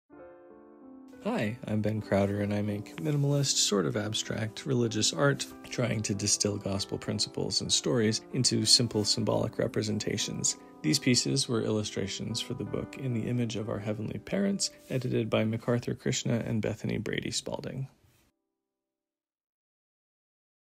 Ben Crowder introduces his art exhibition
In this short video, LDS artist Ben Crowder introduces us to his current show at Writ & Vision in Provo. Crowder's art uses a minimalist style to convey religious principles and stories. These twelve images are illustrations from _In the Image of Our Heavenly Parents: A Couple's Guide to Creating a More Divine Marriage_, edited by McArthur Krishna and Bethany Brady Spalding. The show is on view until March 25, 2023.

Be sure to check out more than 50 minimalist Book of Mormon artworks by Ben Crowder in the Book of Mormon Art Catalog!

Video recording and editing: Aliza Keller and Emma Belnap